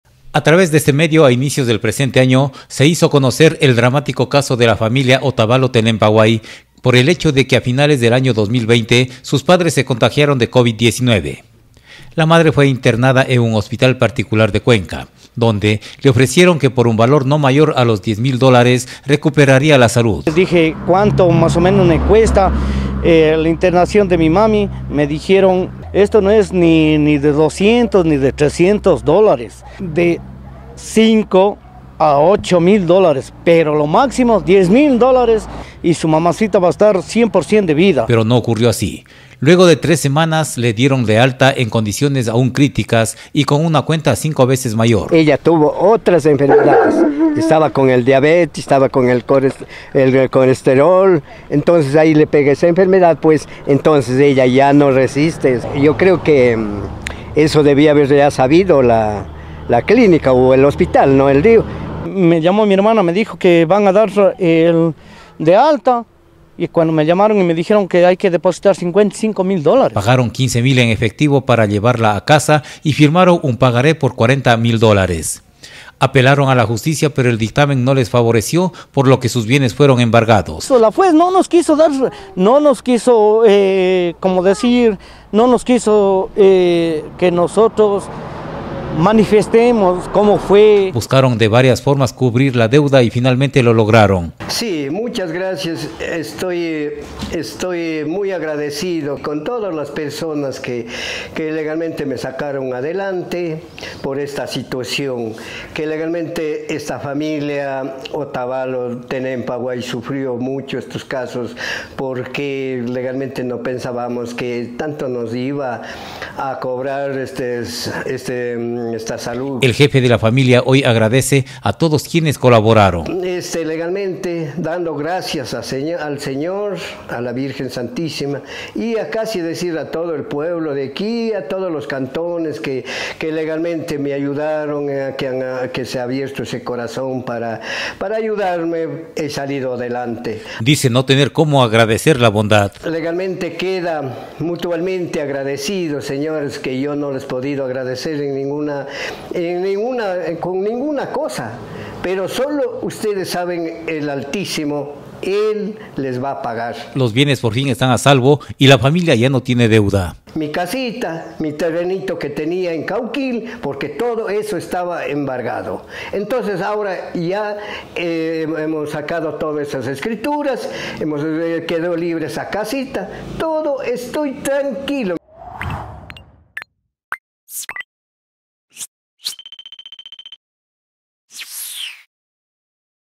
Agradecido con el pueblo de Girón que le ayudó a salvar sus bienes
A inicios de este año se dio a conocer el drama vivido por la familia Otavalo Tenempaguay, cuando quienes la integran, por el ansia de salvar la vida de la madre de hogar que enfermó de covid, estuvieron apunto de perder todos su bienes, al ser embargados por un centro de salud de Cuenca debido al alto costo que representó el internamiento pero que ni siquiera sirvió para salvarle la vida. El jefe de hogar que logró sobrevivir, hoy agradece a la Girón por el apoyo y la ayuda brindada para solventar la deuda.